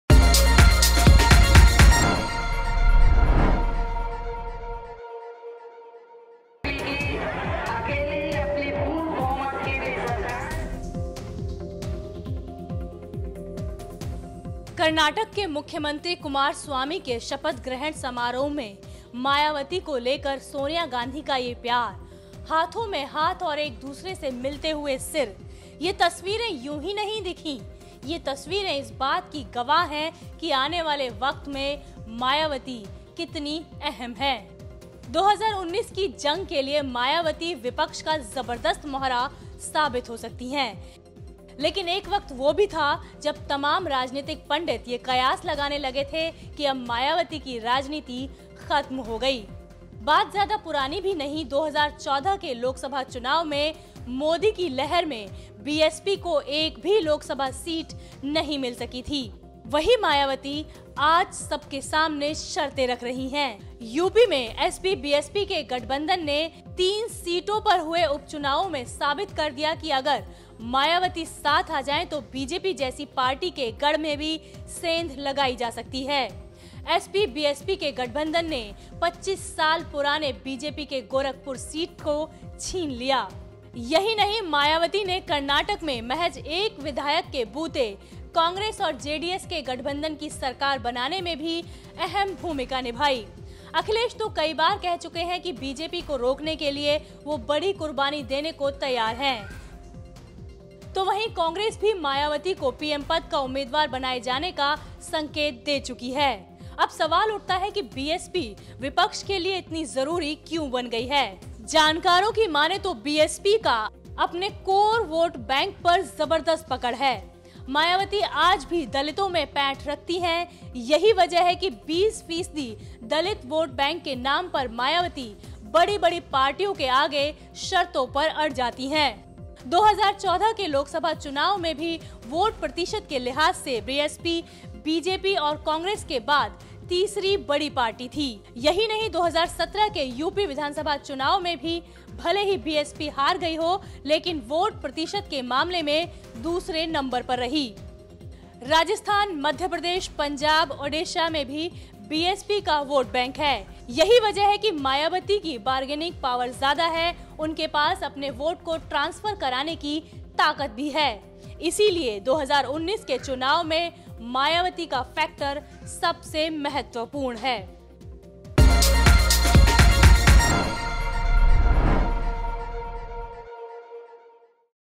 क्या विपक्ष का सबसे मजबूत मोहरा साबित हो सकती है Mayawati, देखिए Report.
2014 के लोकसभा चुनाव में बुरी तरह हारने वाली मायावती आज मोदी के खिलाफ विपक्ष का सबसे बड़ा मोहरा बन गई है...आईए जानते हैं कि आखिर मायावती में ऐसा क्या है जिसके सहारे विपक्ष बीजेपी को मात देने की योजना बना रहा है..